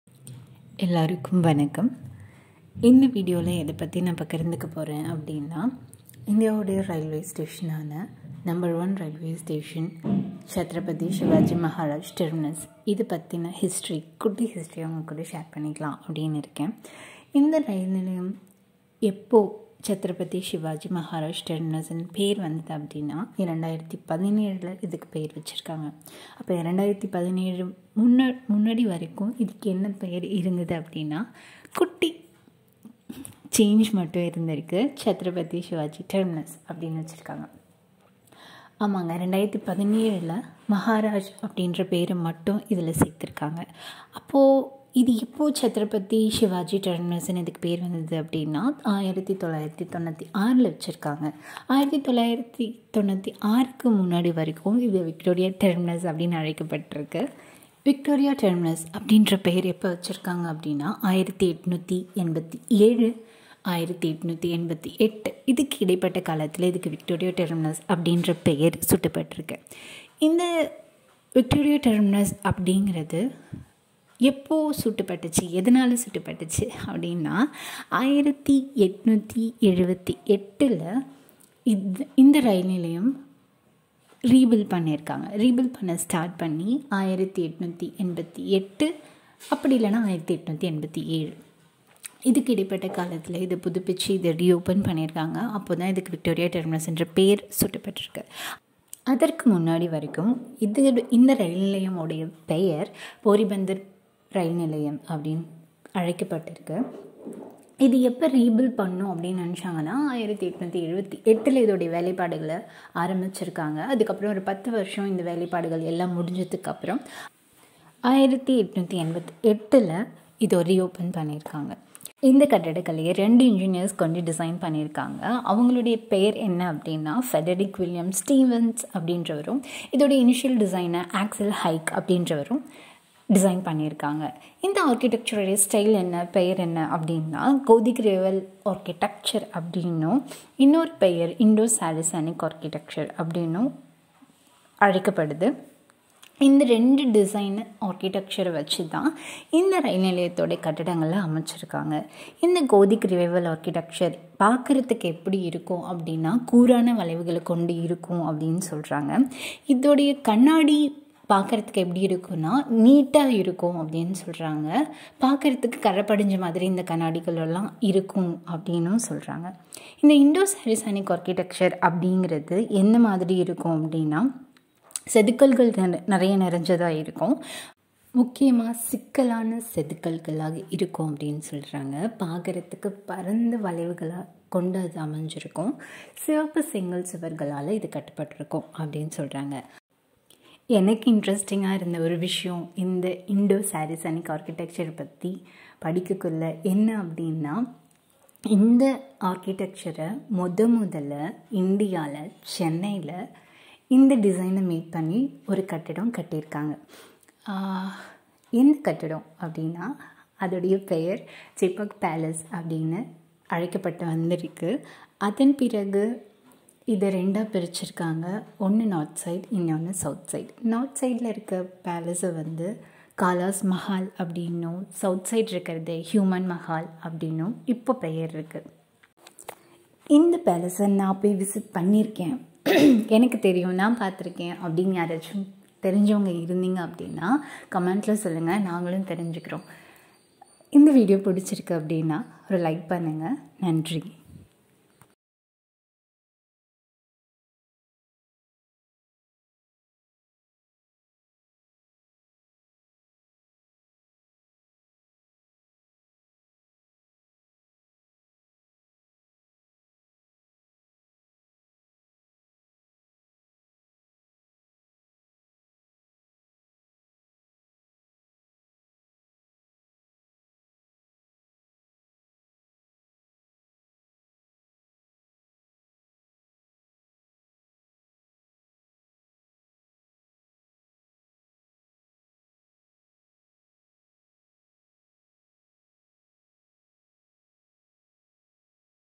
Chepauk palace | interesting History of CSMT
Small history of CSMT
First Indo saracenic architecture buil in chennai.